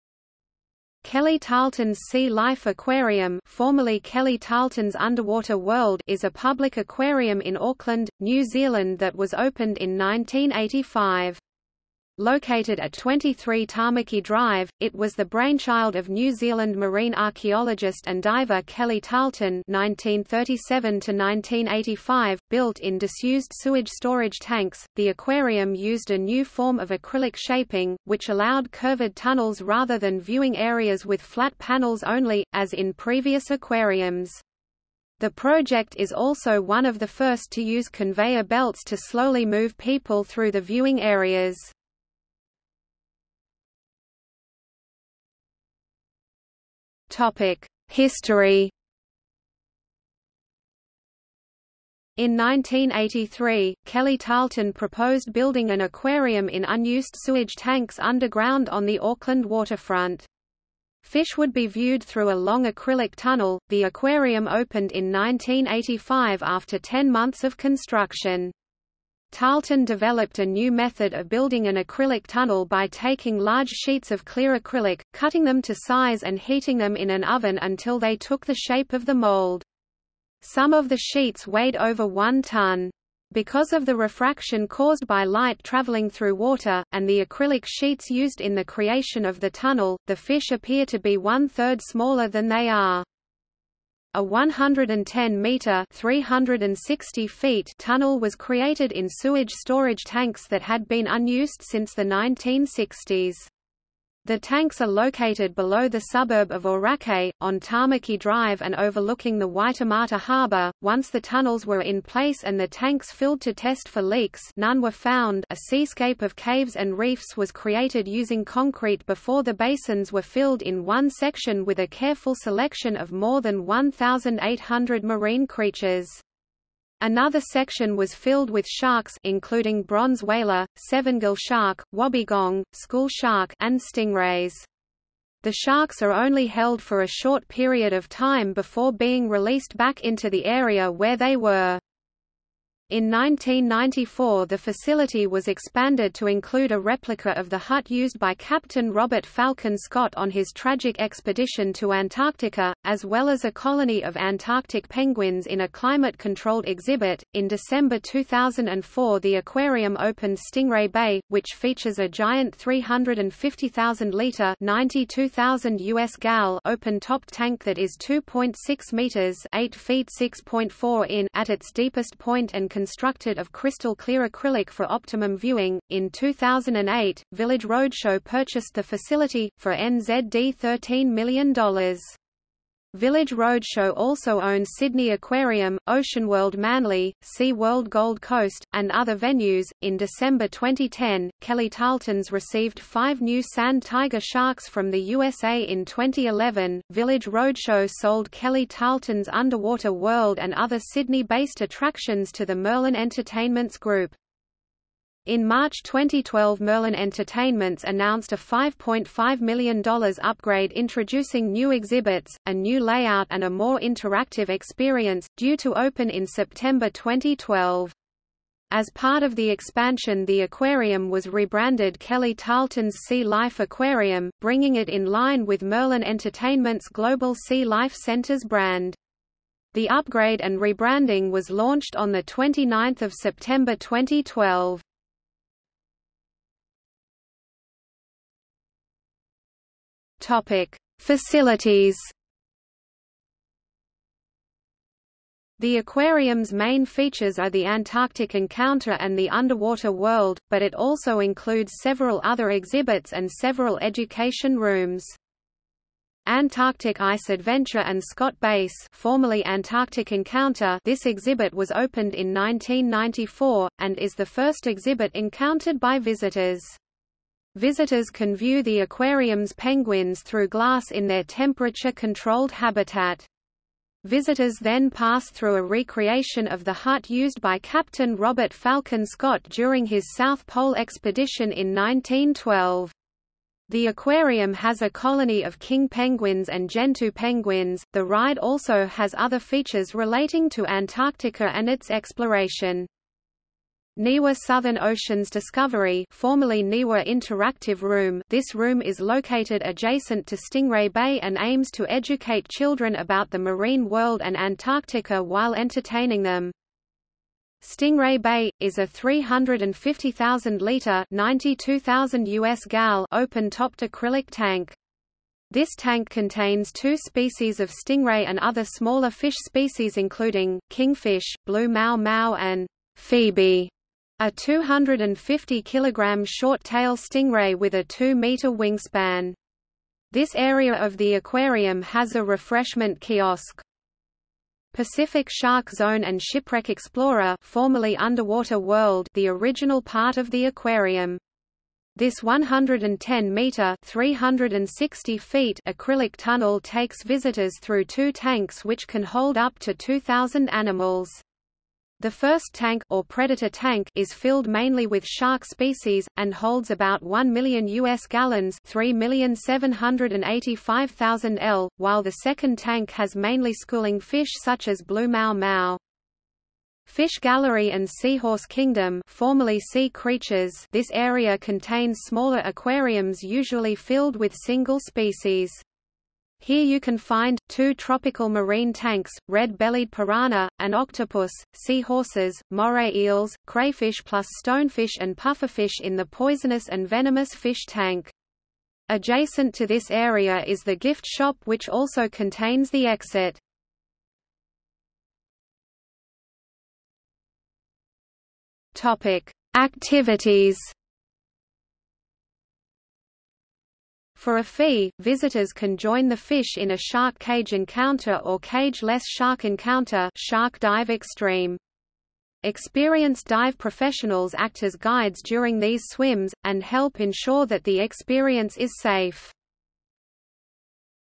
Kelly Tarlton's Sea Life Aquarium | Wikipedia audio article
00:00:43 1 History
00:03:56 2 Facilities
00:06:35 3 Activities

- Socrates

SUMMARY
Kelly Tarlton's Sea Life Aquarium (formerly Kelly Tarlton's Underwater World) is a public aquarium in Auckland, New Zealand that was opened in 1985. Located at 23 Tamaki Drive, it was the brainchild of New Zealand marine archaeologist and diver Kelly Tarlton (1937–1985).Built in disused sewage storage tanks, the aquarium used a new form of acrylic shaping, which allowed curved tunnels rather than viewing areas with flat panels only, as in previous aquariums. The project is also one of the first to use conveyor belts to slowly move people through the viewing areas.